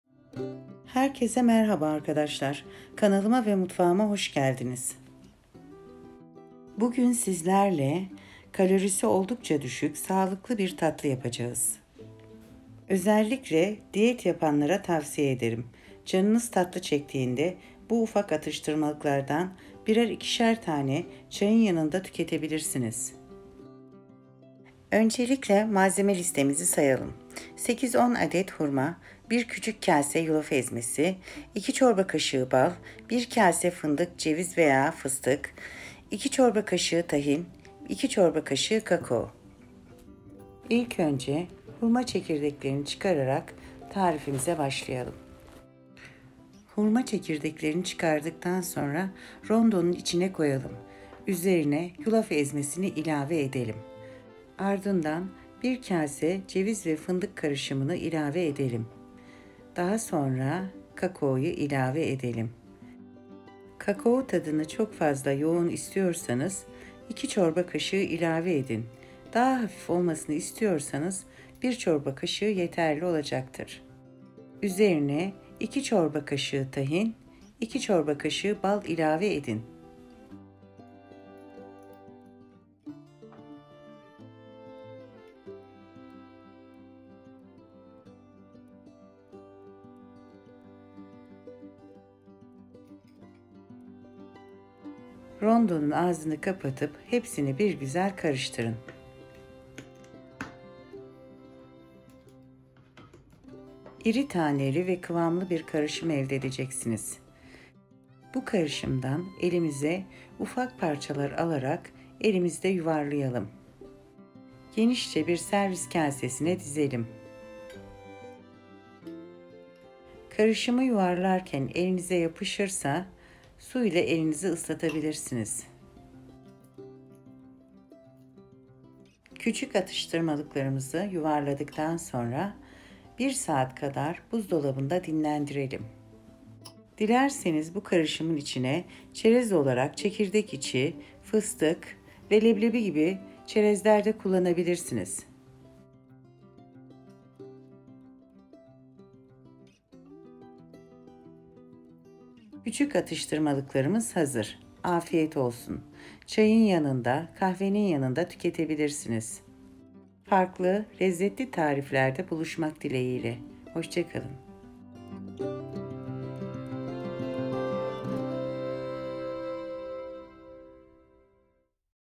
Diyet tatlı. Organik atıştırmalık tarifi
Hafif atıştırmalık
Malzemeler: 8 -10  adet hurma
Küçük bir kase yulaf ezmesi
2 çorba kaşığı bal
2 çorba kaşığı tahin
1 kase ceviz, fındık
2 çorba kaşığı kakao
Yapılışı:
İlk önce hurma çekirdeğini çıkarın rondo nun içine koyun üzerine yulaf ezmesini ￼ ilave edin. Bir kase fındık ceviz karışımını ilave ettikten sonra iki çorba kaşığı bal iki çorba kaşığı tahin ilave edin. İki çorba kaşığı kakao ilave edin.￼ karıştırın. İri taneli bir kıvam aldığı￼ zaman rondo dan çıkarıp elimizde yuvarlaya lım..geniş bir kaseye koyup buzdolabında bir saat dinlendirin. Çayın yanında ikram edin. Afiyet olsun.